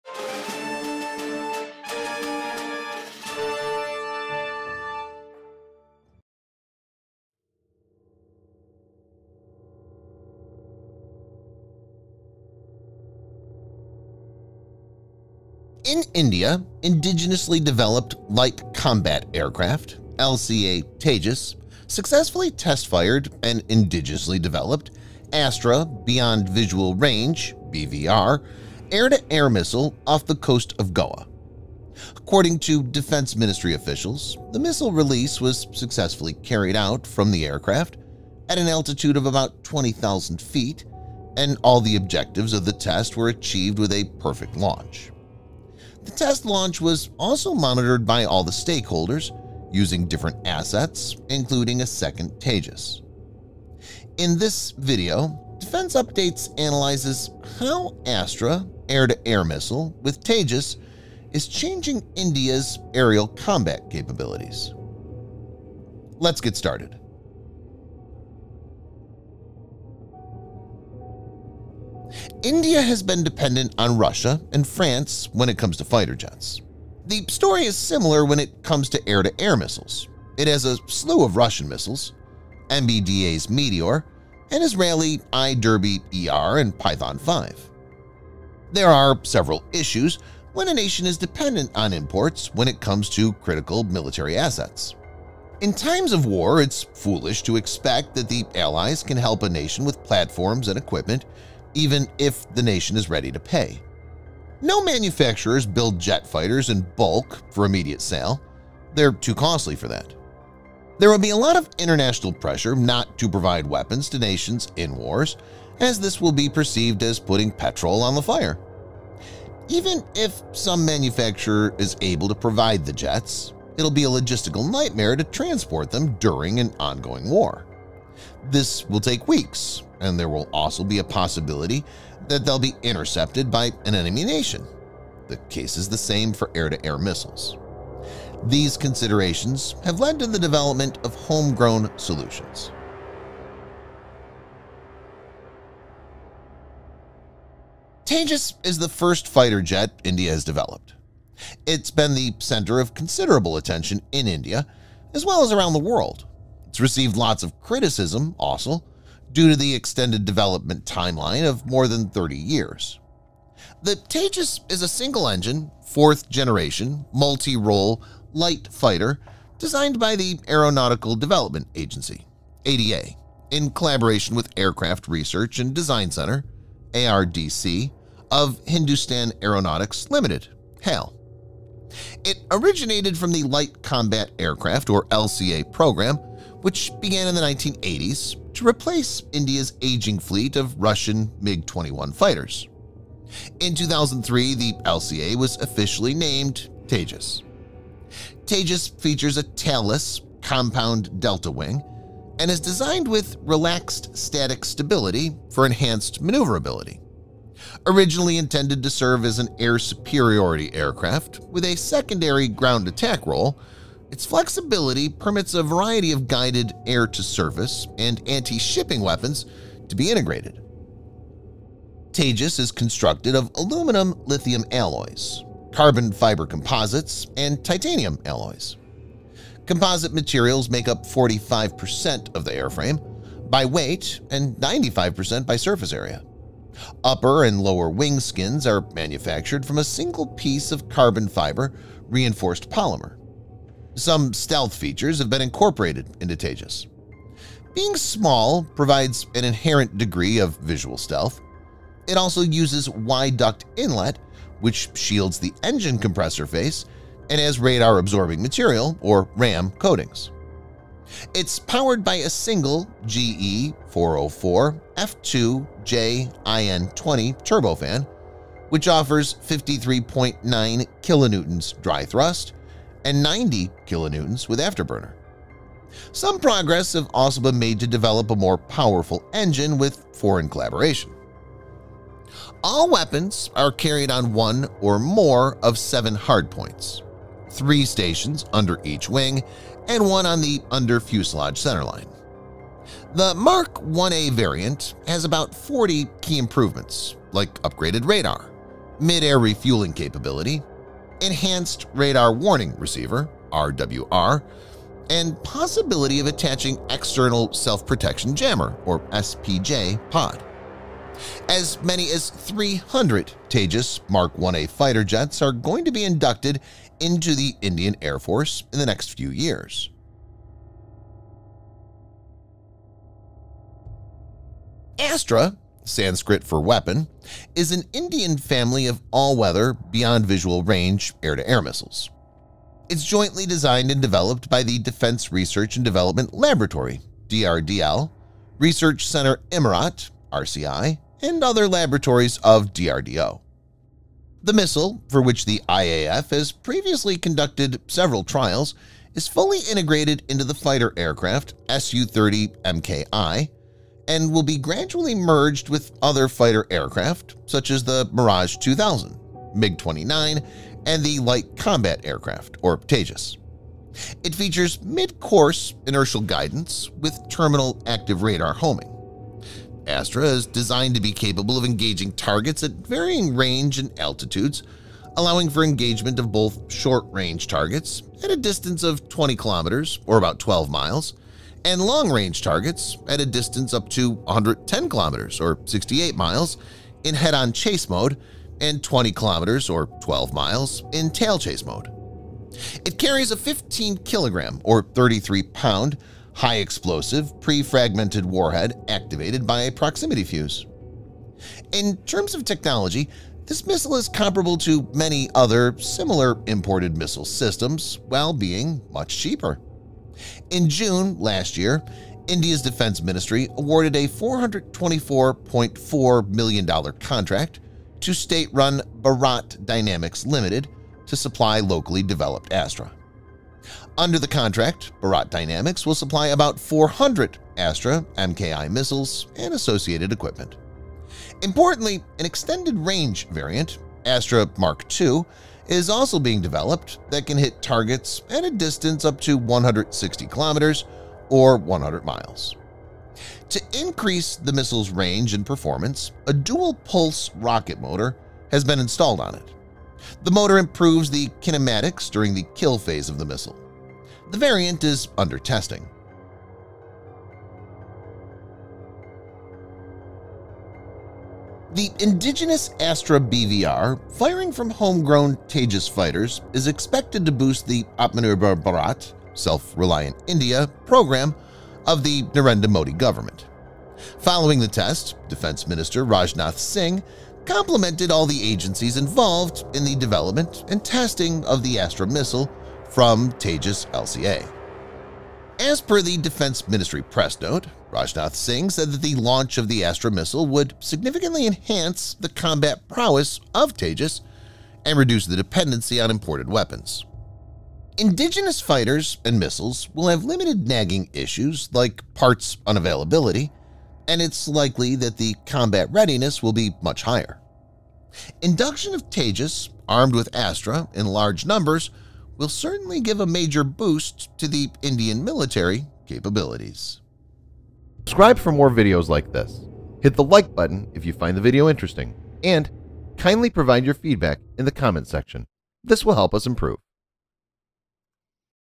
Tejas fighter with ASTRA air to air missile | India's deadly indigenous combo !
In India, indigenously developed Light Combat Aircraft (LCA) Tejas  successfully test-fired an indigenously developed ASTRA beyond visual range (BVR) air-to-air missile off the coast of Goa.

According to defence ministry officials, the missile release was successfully carried out from the aircraft at an altitude of about 20,000 feet and all the objectives of the test were achieved with a perfect launch.
The test launch was also monitored by all the stakeholders using different assets including a second Tejas.

In this video Defense Updates analyzes how Astra air to air missile with Tejas is changing India’s aerial combat capabilities ?